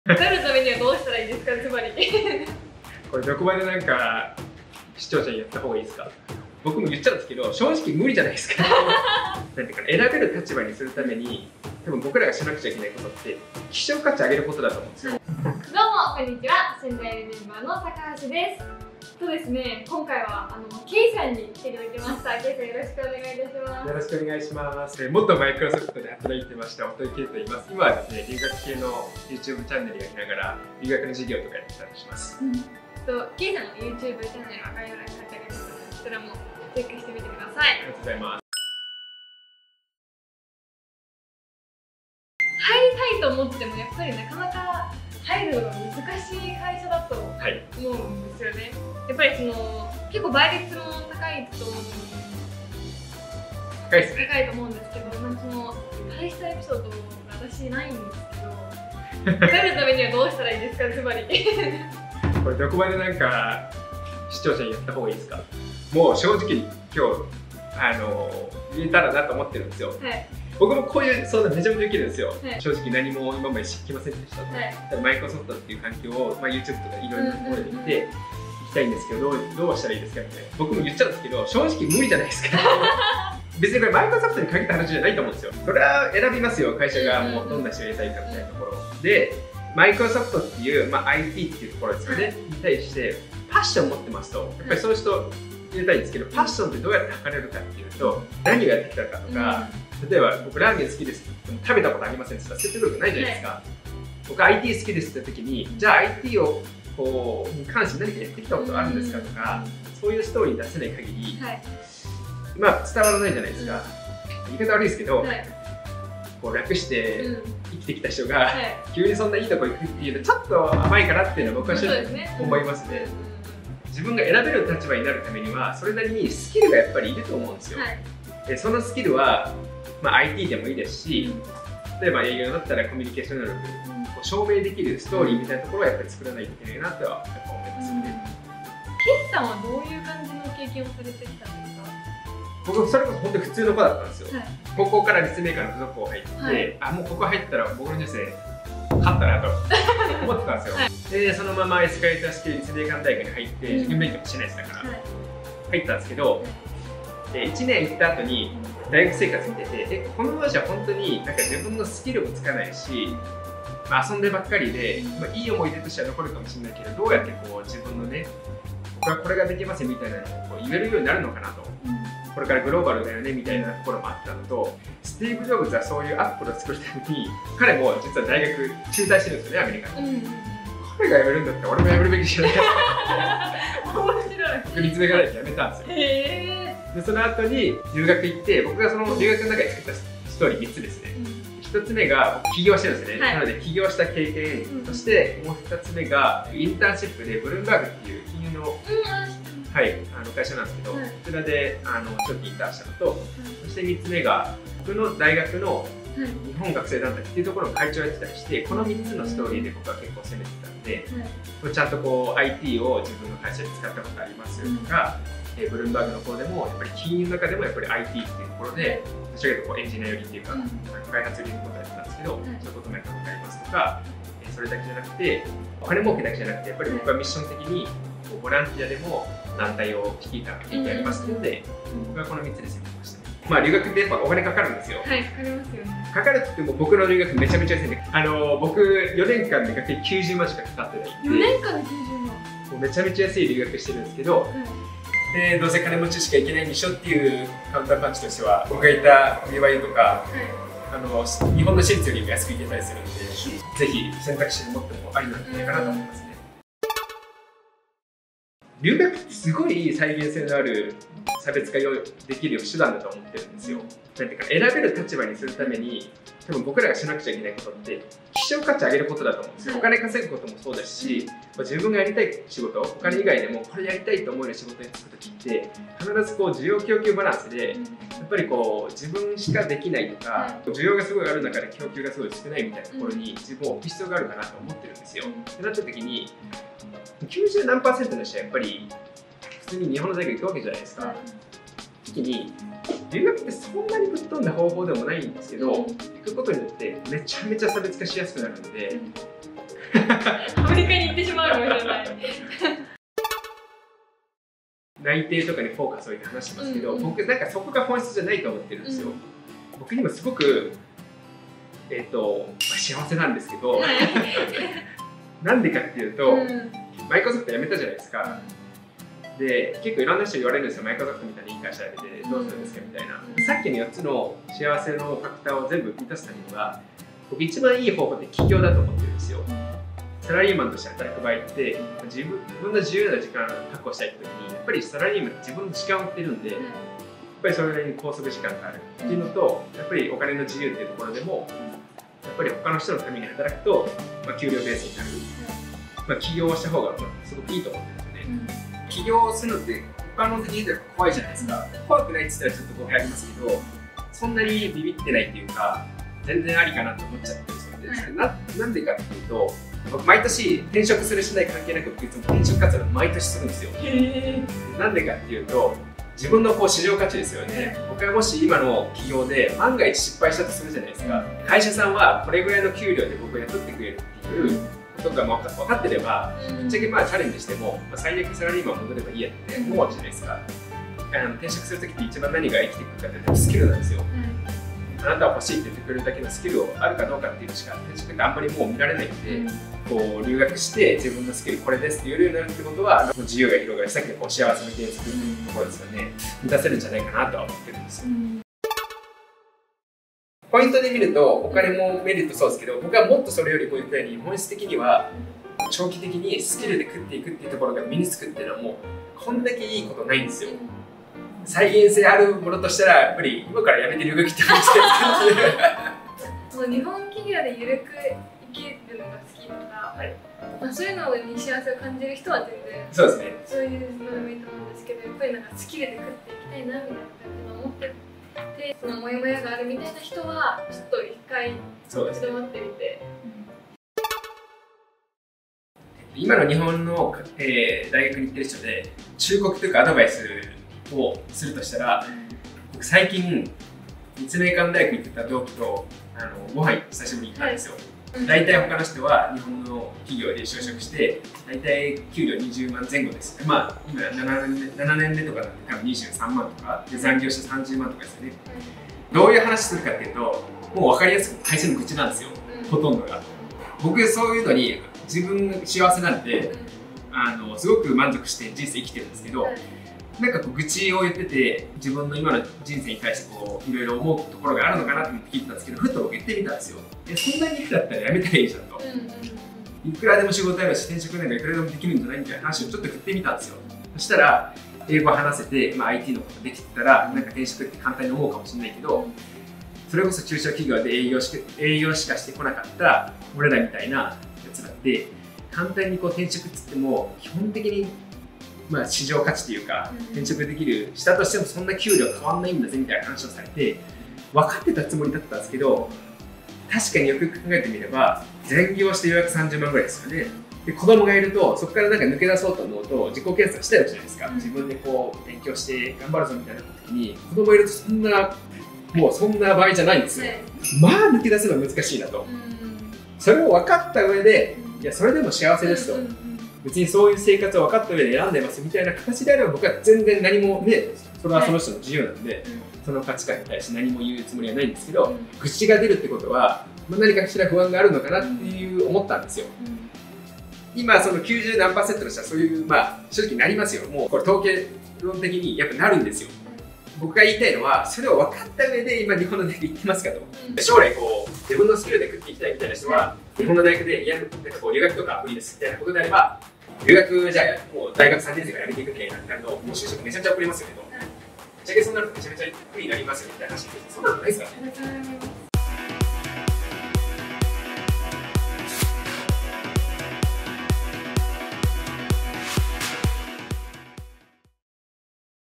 【立命館大生→〇〇→超人気外資】内定のワケ
けいさんのチャンネルはこちら！

👇続きはしゅんダイアリーサイトで公開中！(無料)
👇しゅんダイアリーLINEはこちら！

～～今日の動画～～
今日は立命館学を1年で中退し、マイクロソフトに入社されたけいさんへのインタビュー動画です！ぶっちゃけ発言連発で、超人気企業に入社したい、スキルで食べていけるようになりたいと考える人にとって有益な情報が詰まっているので是非ご覧ください！

【内容】
0:00 ダイジェスト
0:15 企画説明、出演者紹介
0:40 ガクチカ弱い人でも受かる？
1:02 考えが甘すぎる
2:05 スキルが超重要
2:24 学生時代の気づき
2:55 スティーブジョブズ
3:10 3つのストーリー
3:58 お金がなくても留学は可能！？
4:25 今すぐやるべきこと
5:12 会社に縛られないために
6:41 留学で一番のメリット
7:20 日本の大学生に忠告